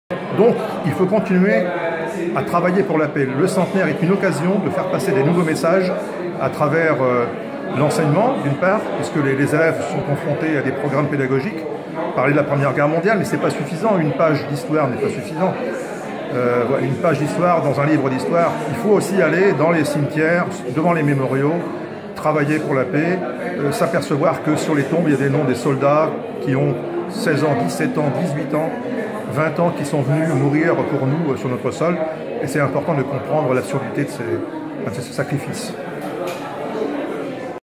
Yves Potard, médaillé du centenaire de la Grande Guerre dans la Somme
Yves Potard, président de l'association The Digger Cote 160, a reçu la médaille départementale du centenaire de la Grande Guerre le 14 novembre 2017.